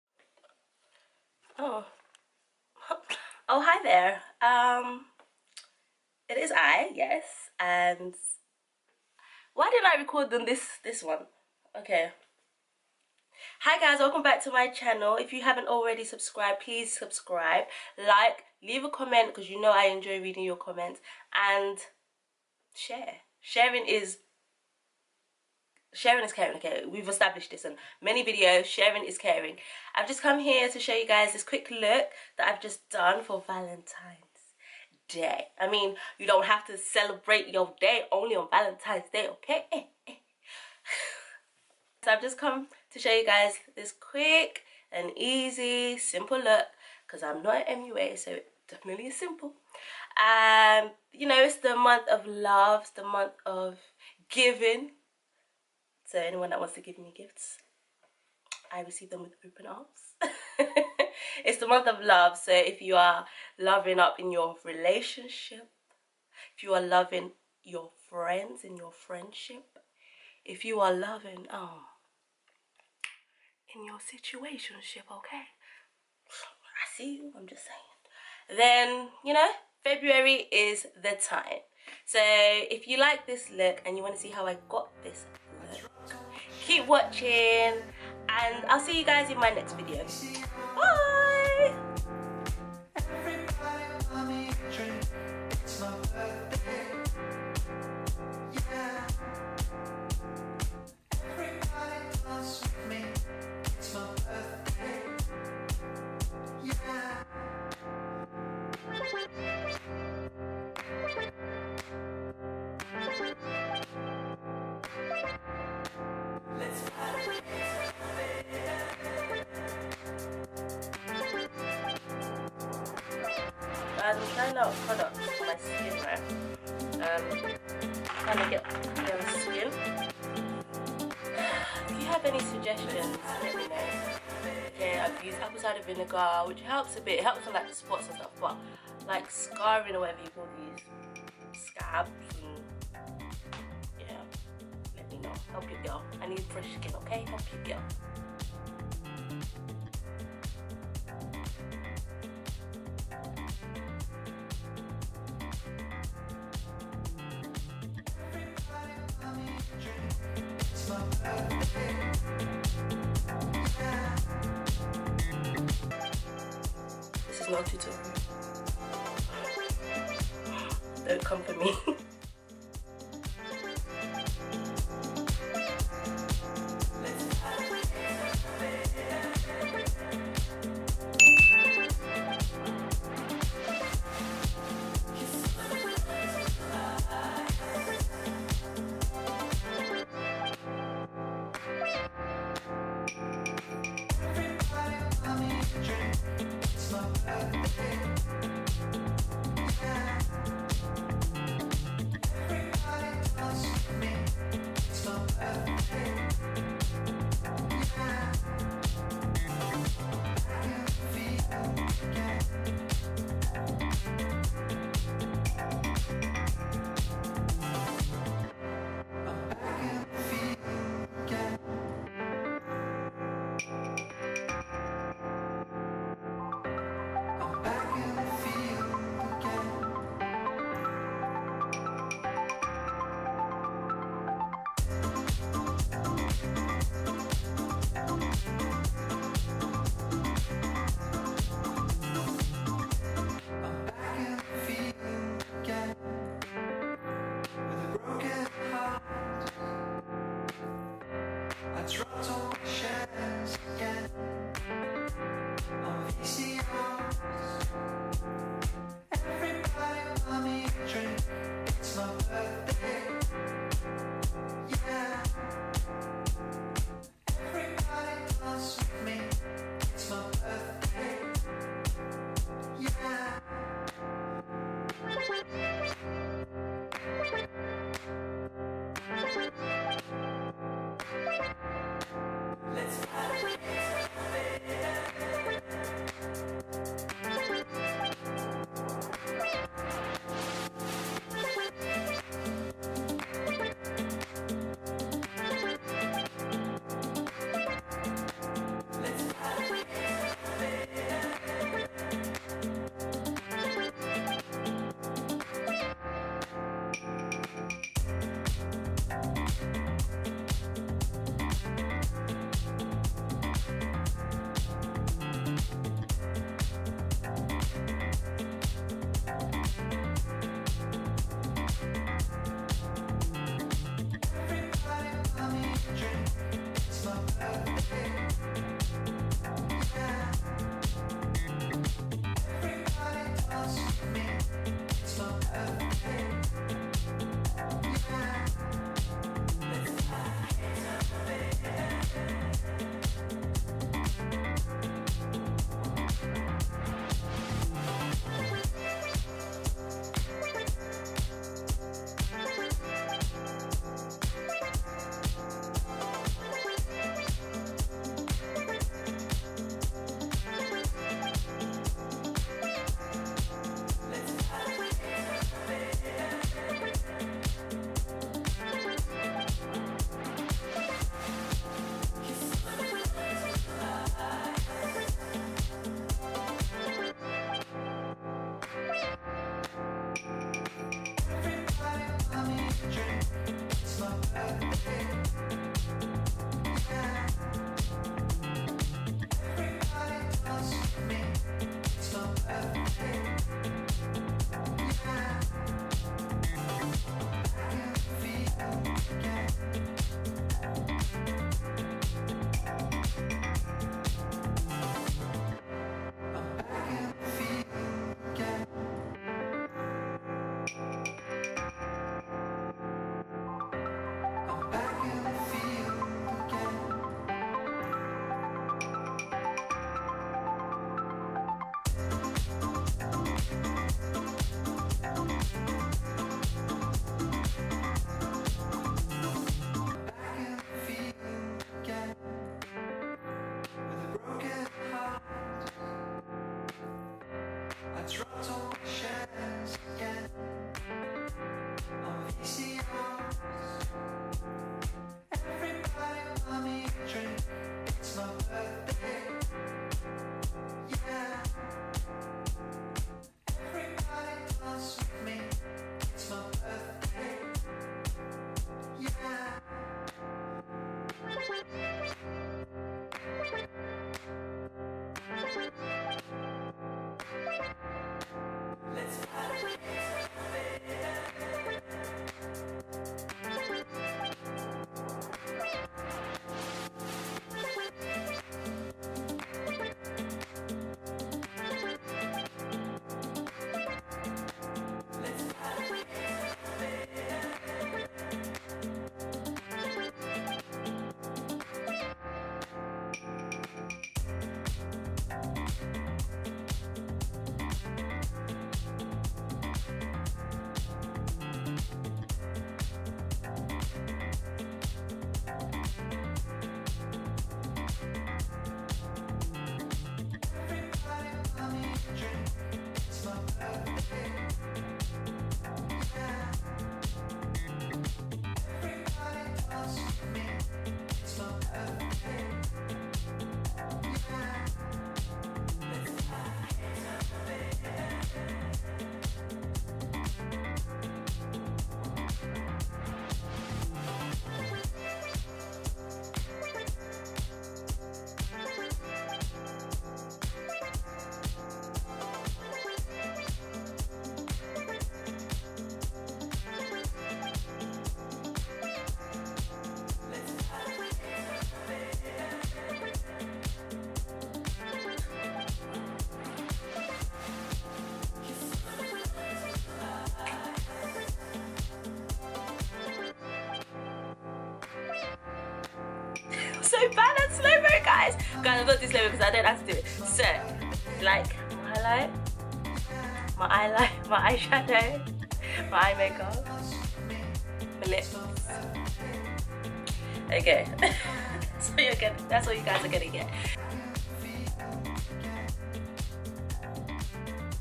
GRWM VALENTINES MAKE UP LOOK
❤It’s FEBRUARY the month of LOVE. Showing how I created this look, just showing not teaching 😊 Happy viewing love bugs.